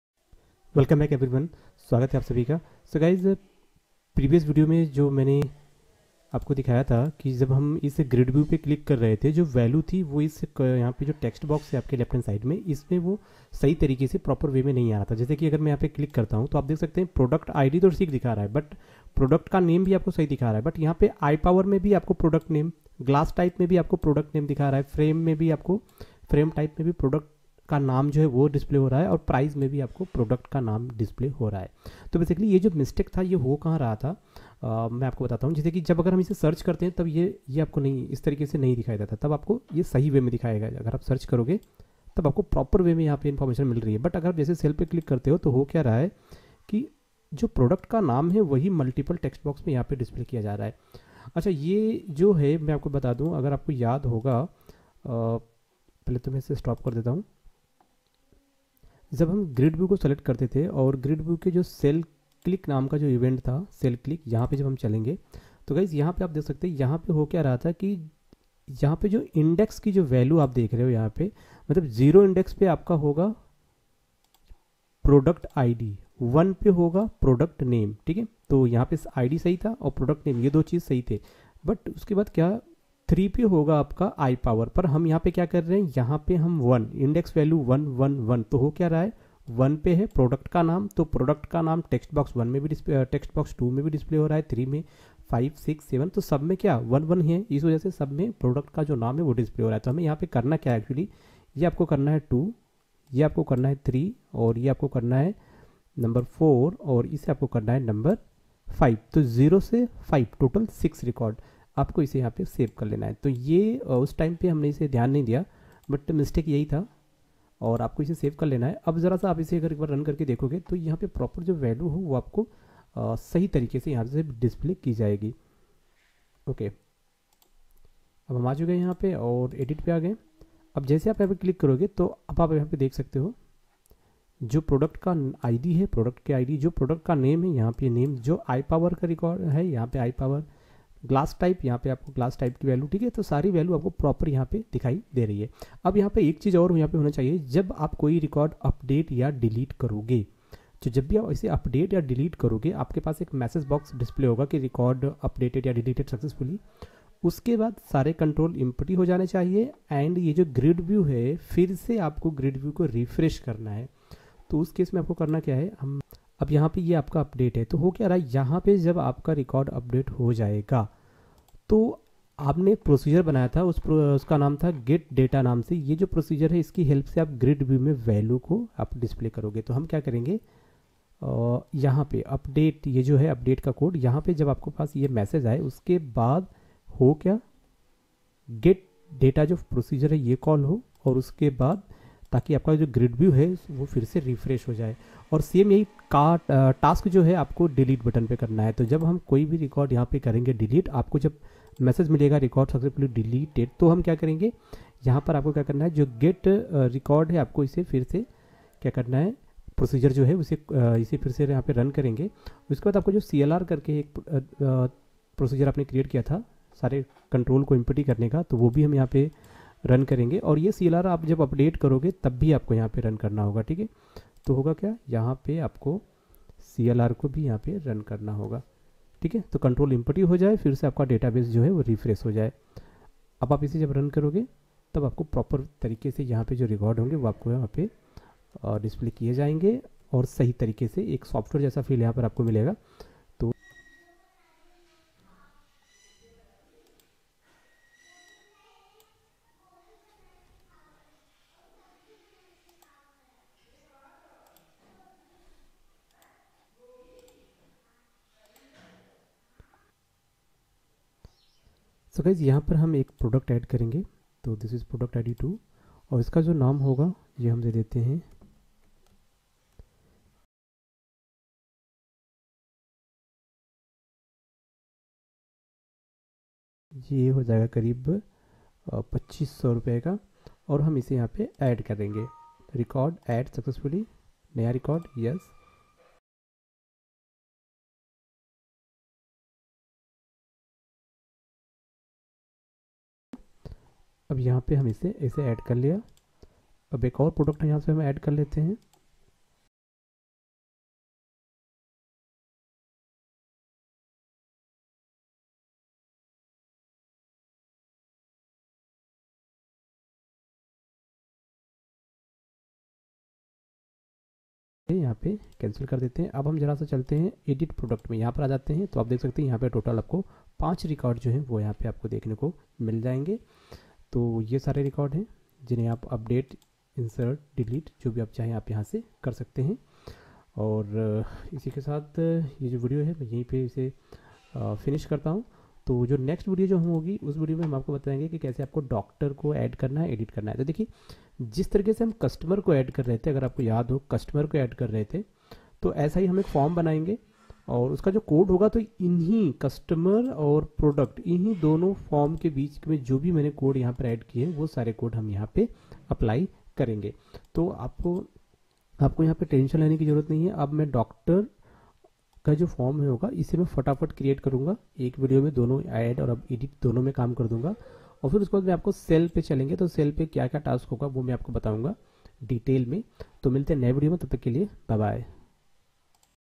VB.NET Optical Shop Project | DataGridView Cell Click Error Solution | Part-14
VB.NET Optical Shop Management System Project | DataGridView Cell Click Error Solution Video|  Part-14

How to create Optical Shop Management System in VB.NET with MS Access Database.

Original Credits: who made this video CODERBABA and YOUTUBE.

GOOD LUCK